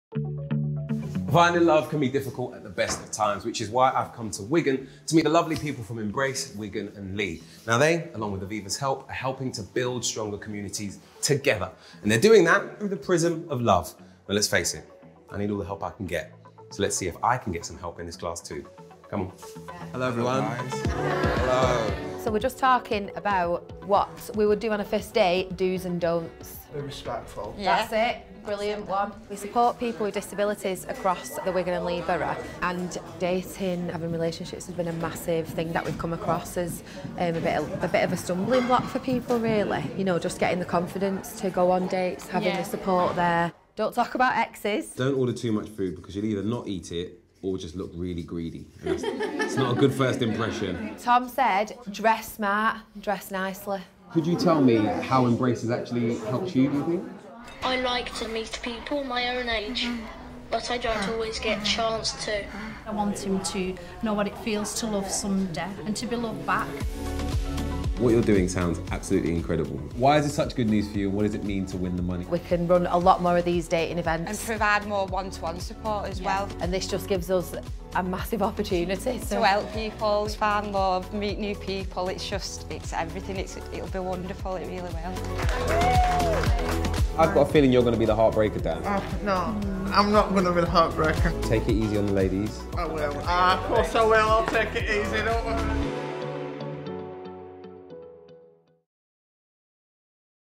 Reggie Yates visits Embrace Wigan & Leigh for Aviva!
Reggie Yates, Aviva Community Fund ambassador, paid a visit to Embrace Wigan & Leigh and shared the great news that they had won one of this year's awards.

Client: Aviva
Agency: MHP Communications
Copyright: Aviva